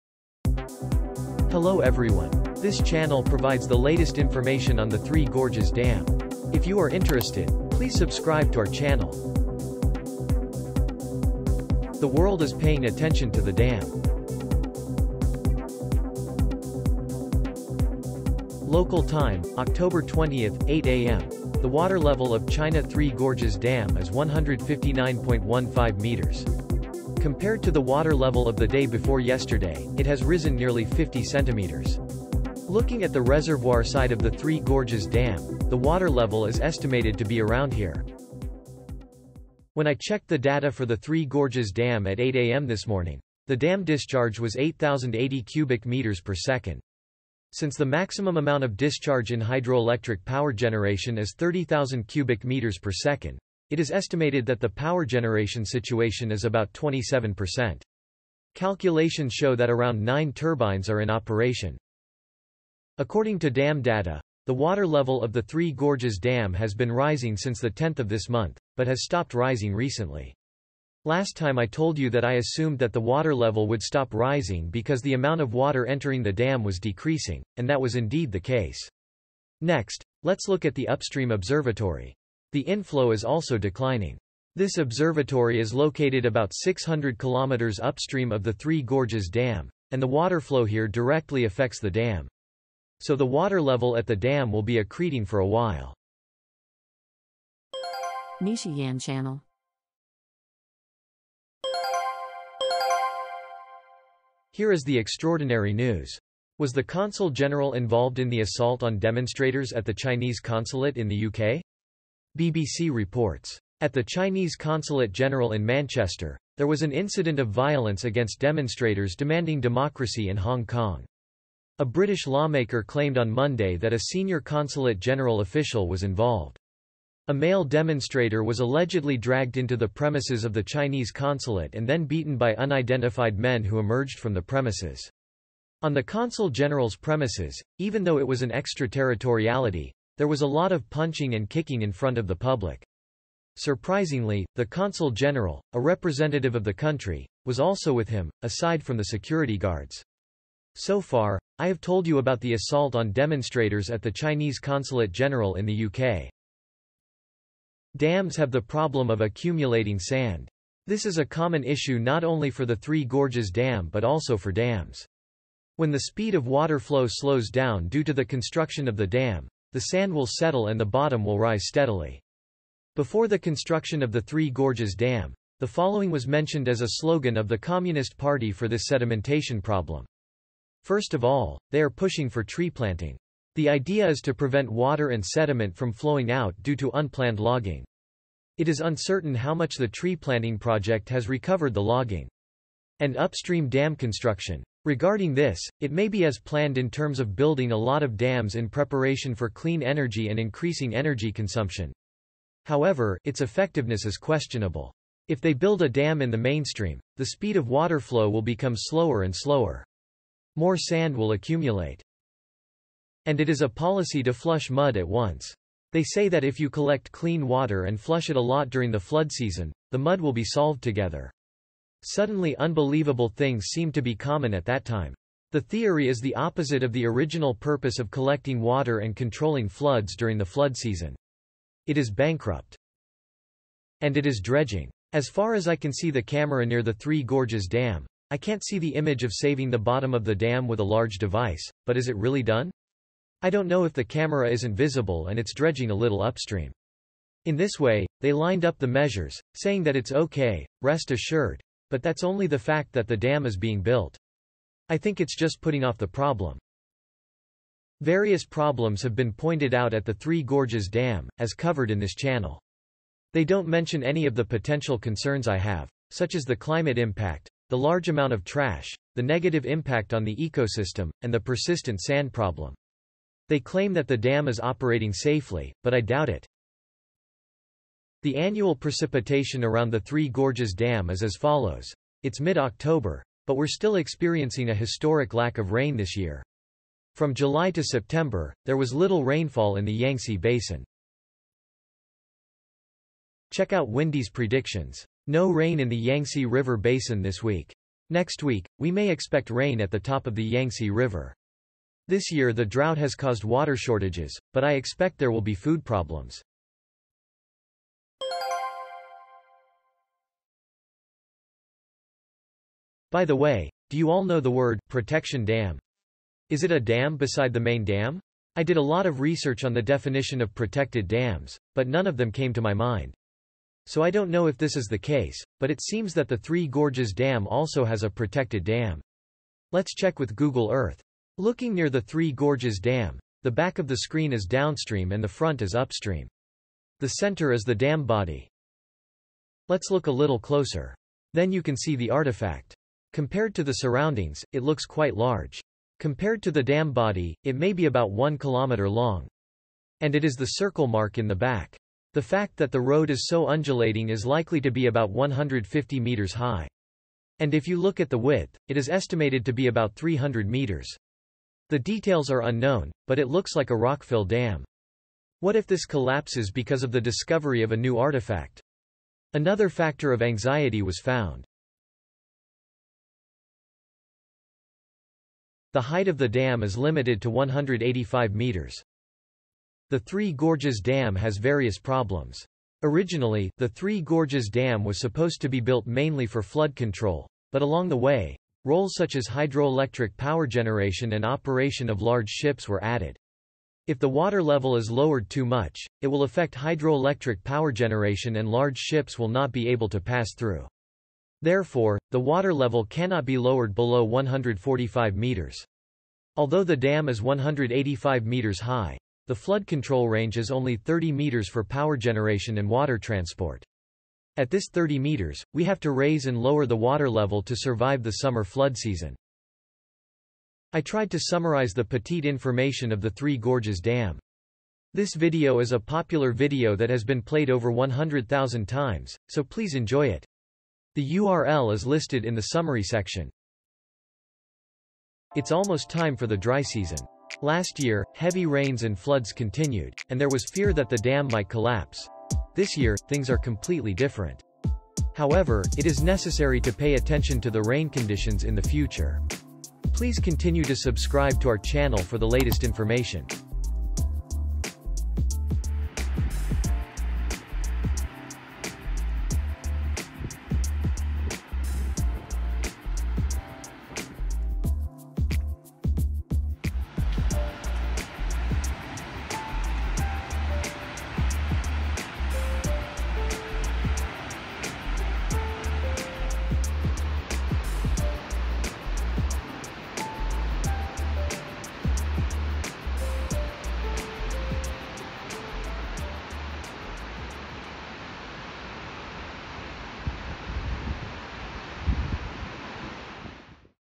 China Three Gorges Dam ● Full of Sand ● October 20, 2022 ● Water Level and Flood
China's Three Gorges Dam, known as one of the world's largest dams, has been hit by a drought this year that has significantly reduced runoff. Although it is not reported in the mass media, the problem of sand deposits in dams is serious. The dam will cost him $28.6 billion to build and a lot more to maintain. From various data, I think the Three Gorges Dam is dangerous this year.

✅ Related Article

◆Information
damdata